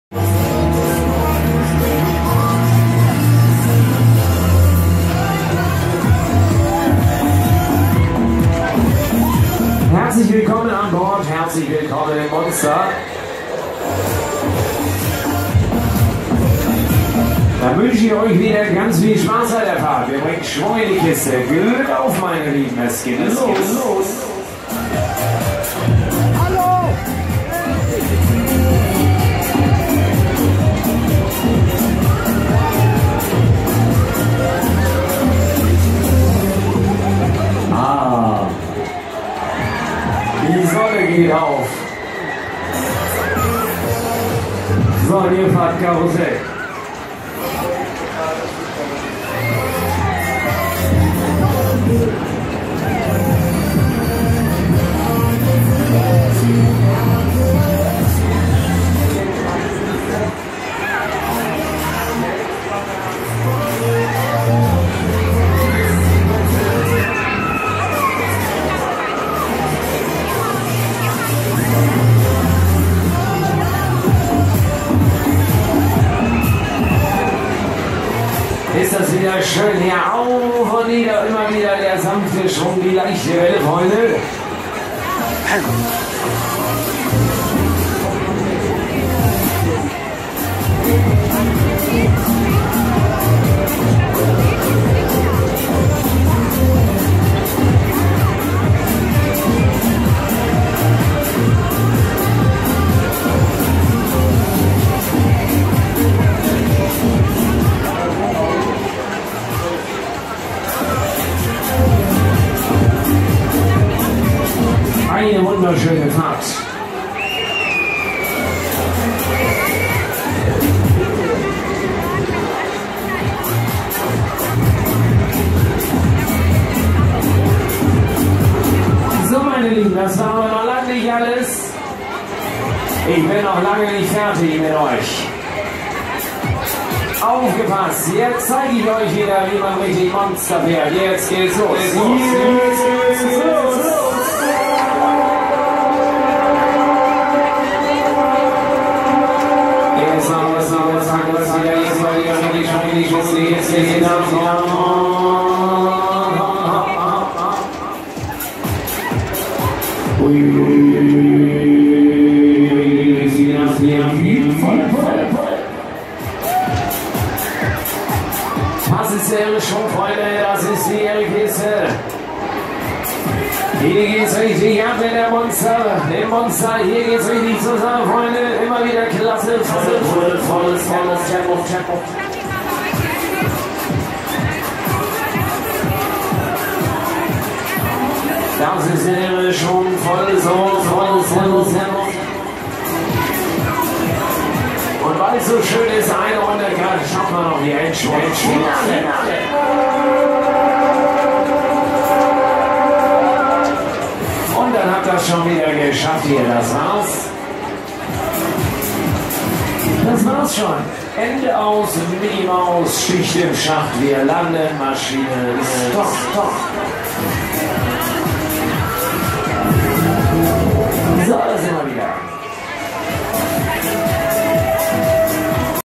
BIG Monster Krameyer Cranger Kirmes Herne 2022
BIG MONSTER von Krameyer auf der Cranger Kirmes in Herne
ein sehr schönes Fahrgeschäft ;) aufnahme vom 12.8